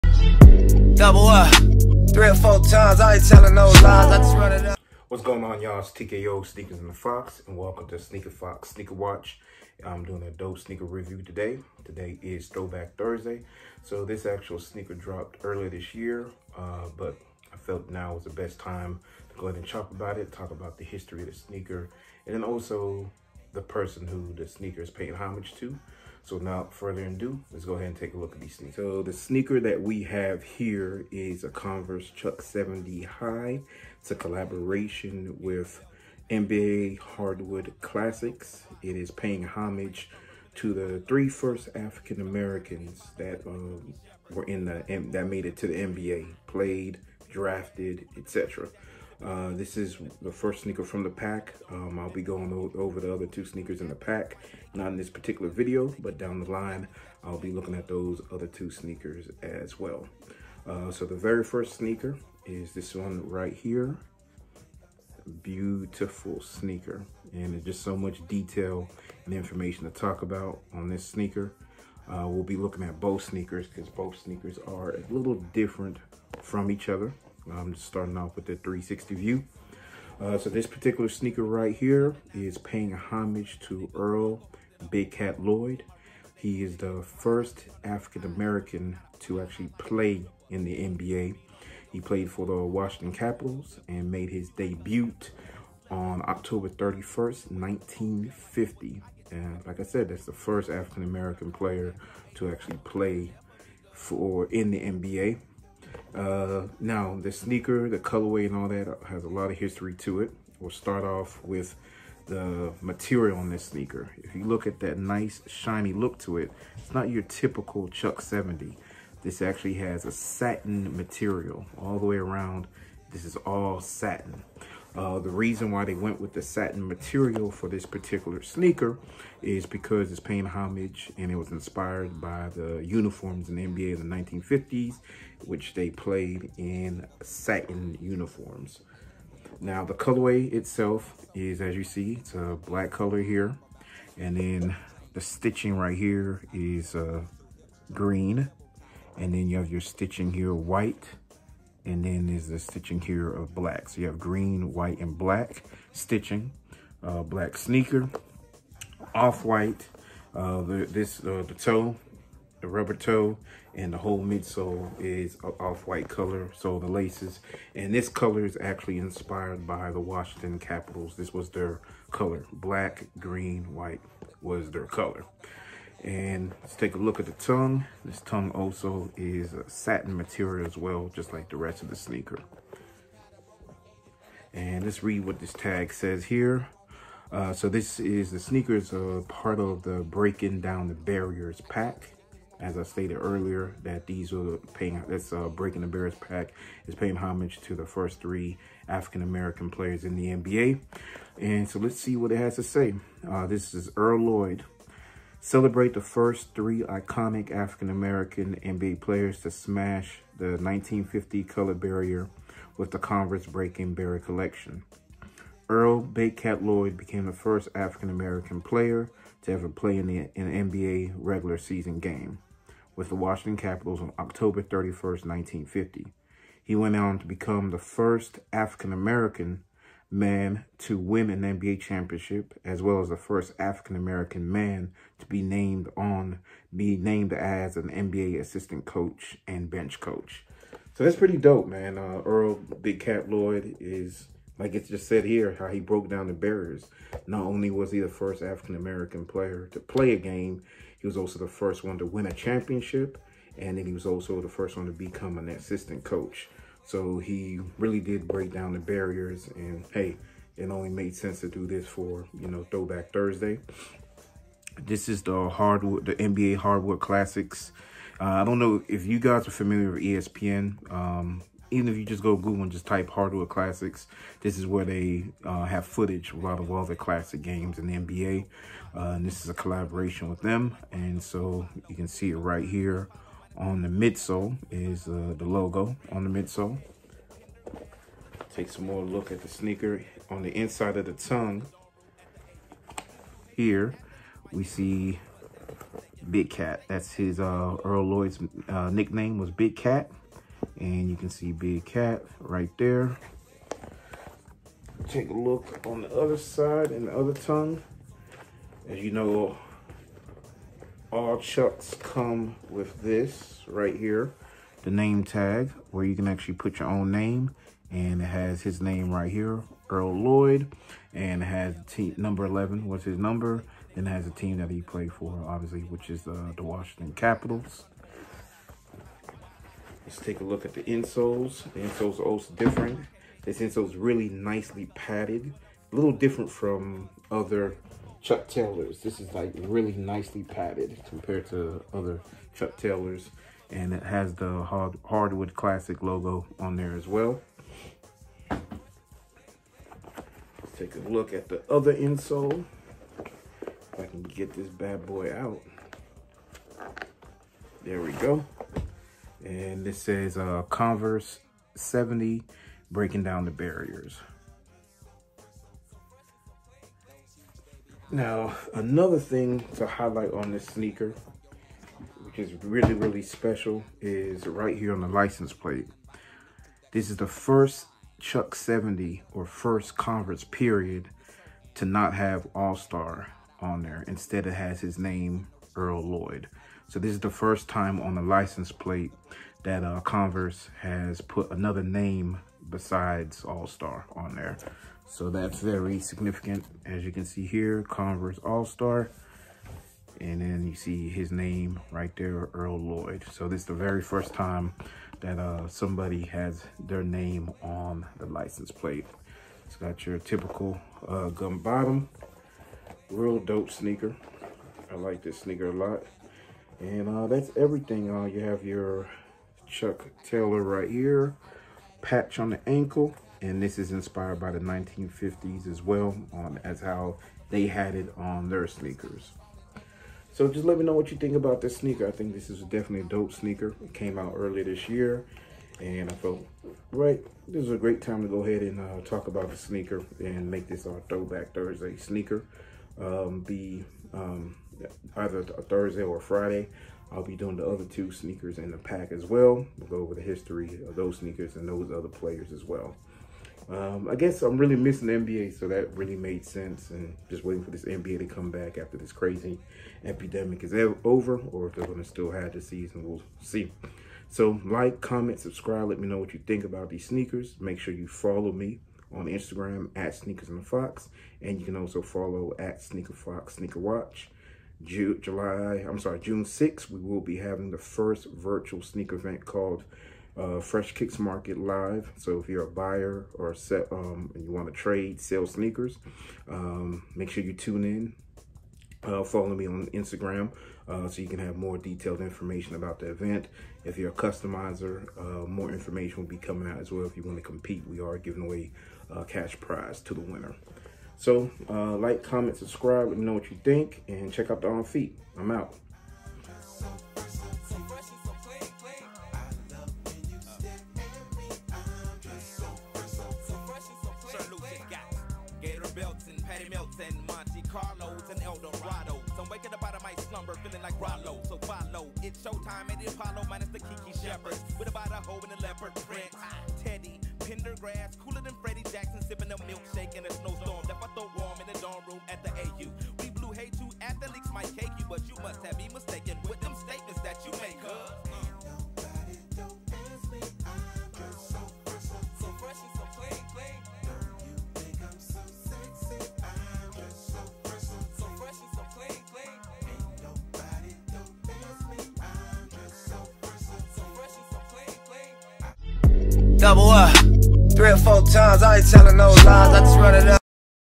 Throwback Thursday: Converse x Hardwood Classic NBA Chuck 70 “Earl Lloyd (1st Black NBA Player)
From the Breaking Barriers pack. Earl “Big Cat” Lloyd first African American to play a game, win championship, and coach in the NBA.

Check us out  @sneakersandafox @sneakerfoxsneakerwatch.

Sneaker Custom Competition for $500 x FKM Live Buy Sell Trade June 6.